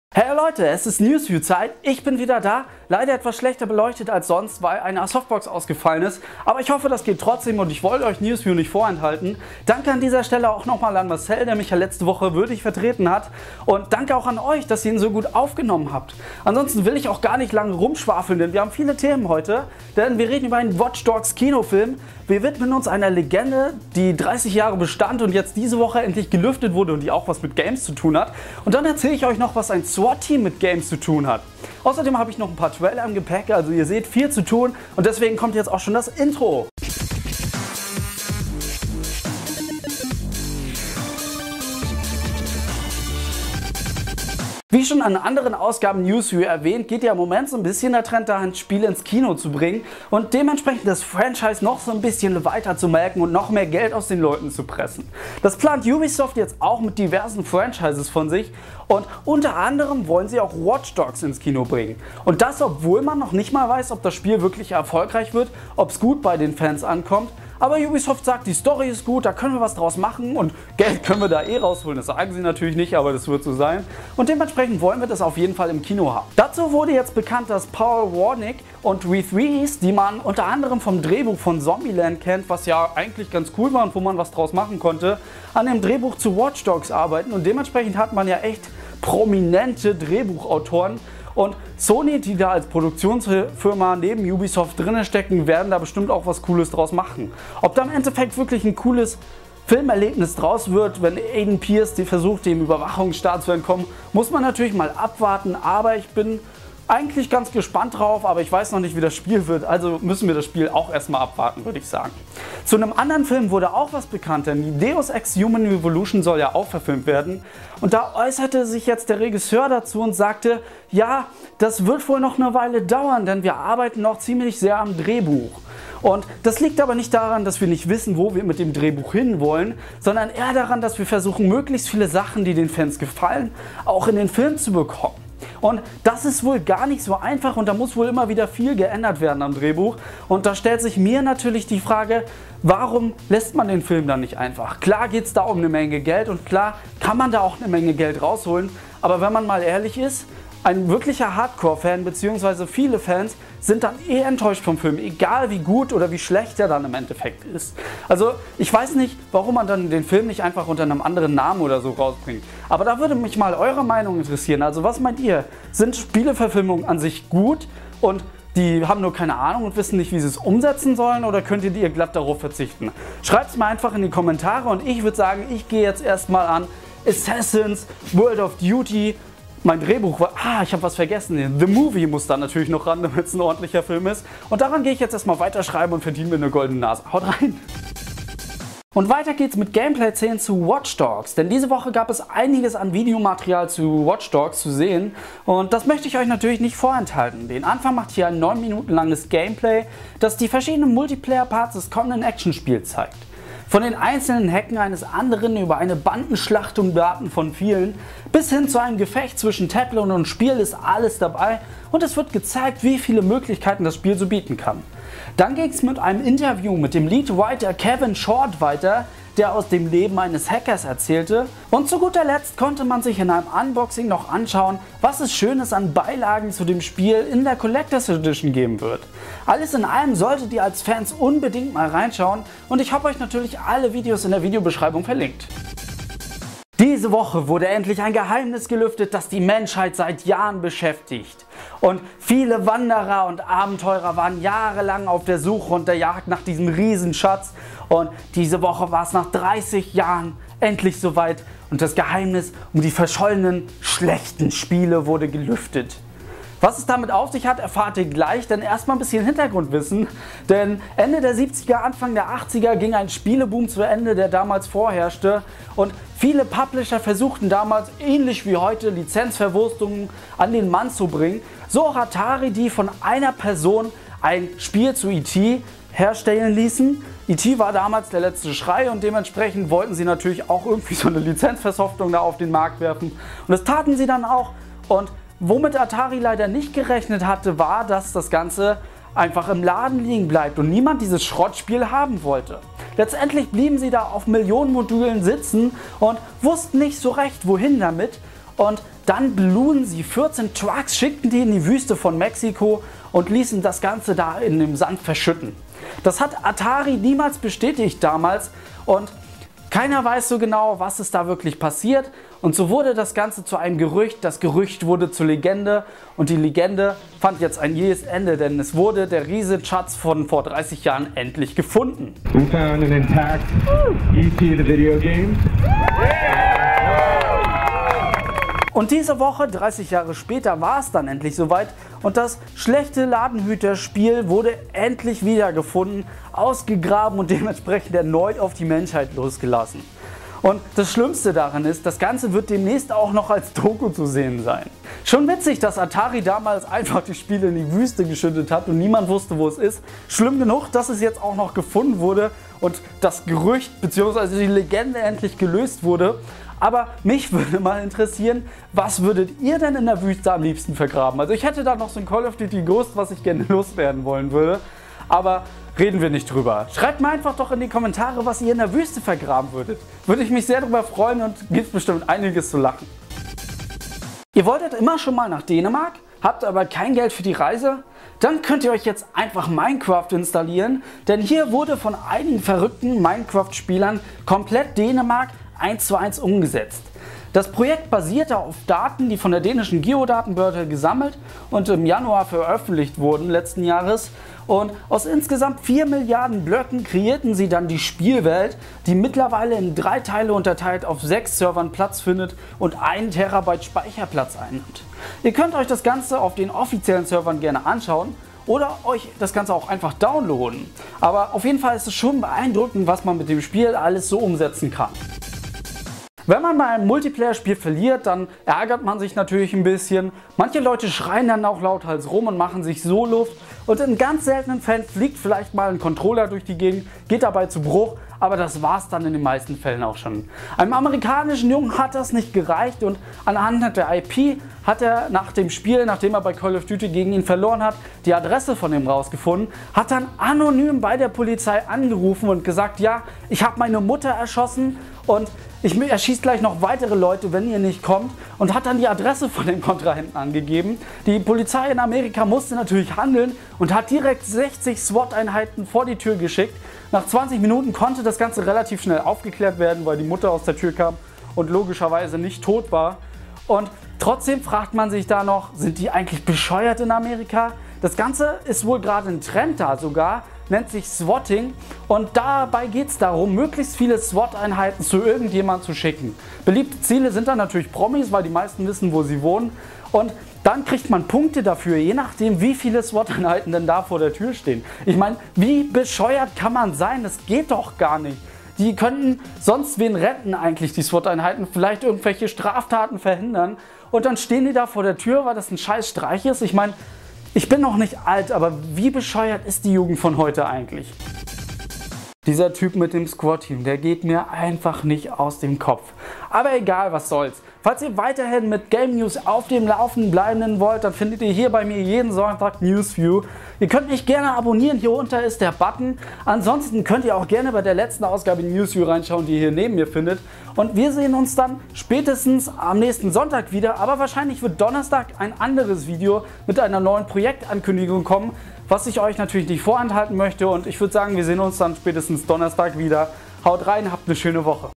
SPIELEMYTHOS NACH 30 JAHREN GELÜFTET - WATCH DOGS KINOFILM GEPLANT - NewsView #19
► NewsView #19
► Spielemythos nach 30 Jahren gelüftet - Watch_Dogs bekommt Kinofilm
► Schlechter Verlierer schickt Squad-Team - Trailer
► Hier präsentiere ich euch die News der Woche